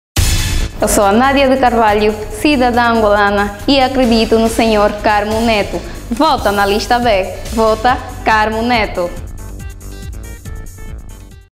Vota em Carmo Neto - Lista B
Caro efectivo da polícia Nacional…!

Em setembro o destino da sua vida pode ganhar um outro rumo com o seu voto na lista “B” Carmo Neto…!